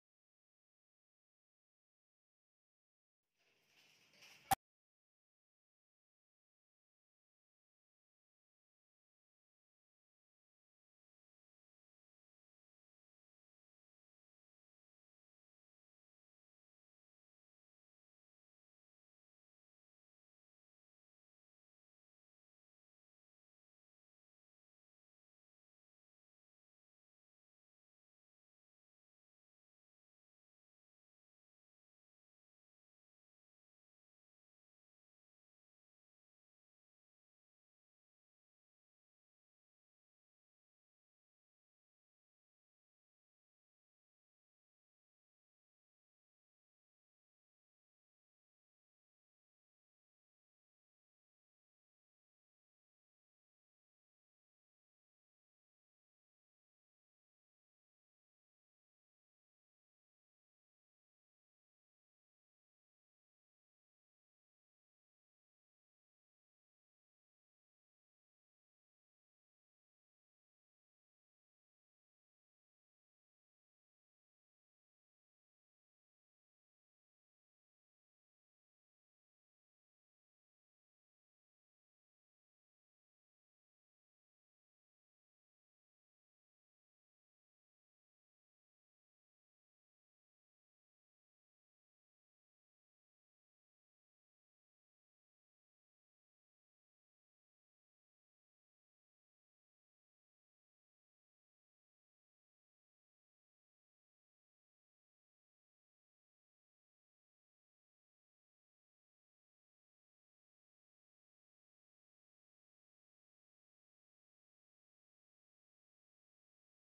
Easy oreo ice cream with 3 ingredients/yummy and tasty😍/
Oreo biscuit  3pkt
Sachet dream whip 2
Milkmaid 1/2 cup